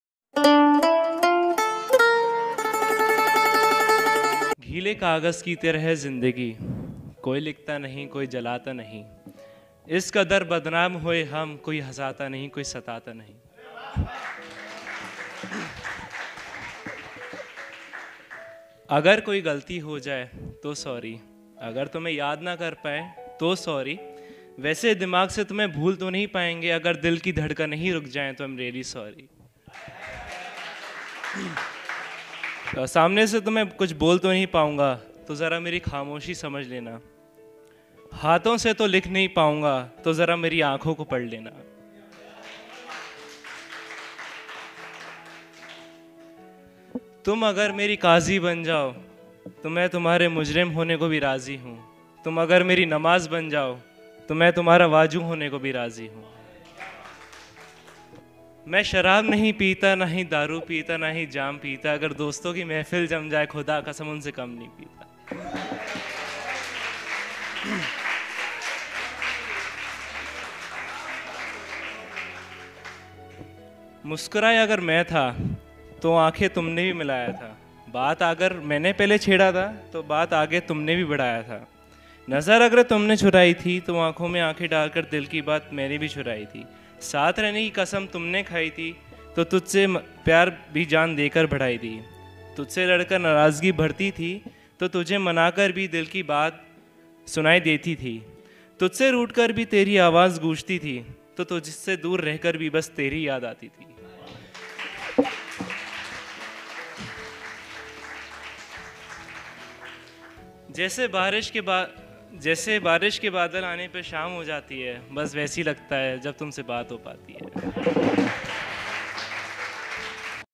Likh Nhi Paunga Tum Aankhe Padh Lena | Srajan Agarwal | Kuch Khaas | Open Mic
Performer name - Srajan Agarwal